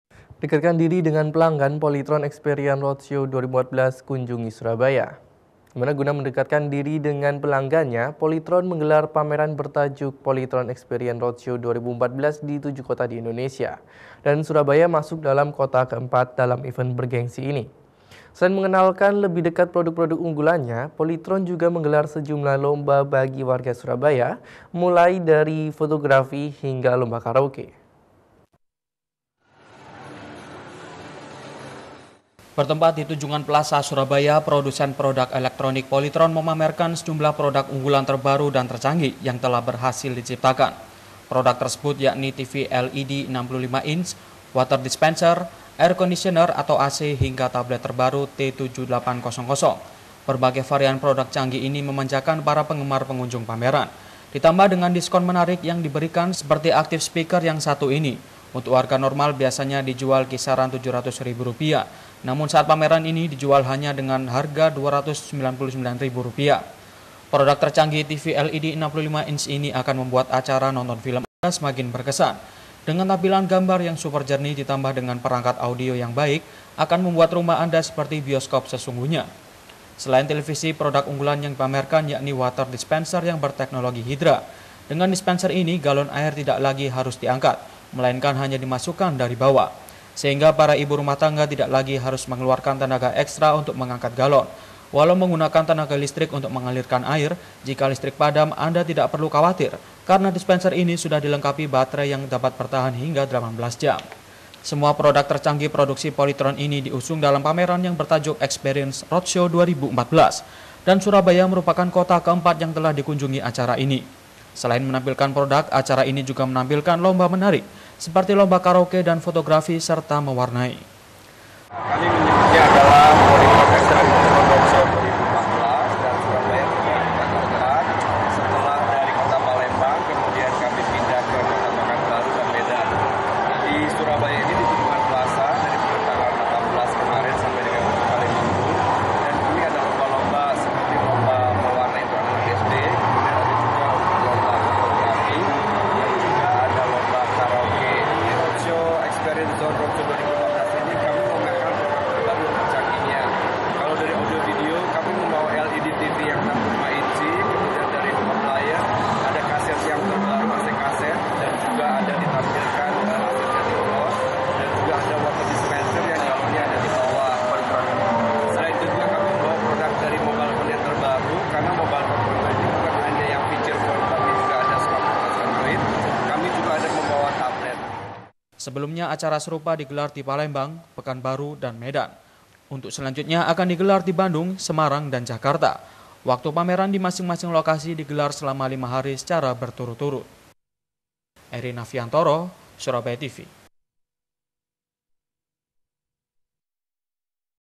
POLYTRON EXPERIENCE ZONE 2014, SURABAYA: LIPUTAN SURABAYA TV
Beragam program promo dengan berbagai kegiatan seperti lomba menyanyi, lomba mewarnai, dan lomba fotografi di dalam acara Polytron Experience Zone 2014, Surabaya.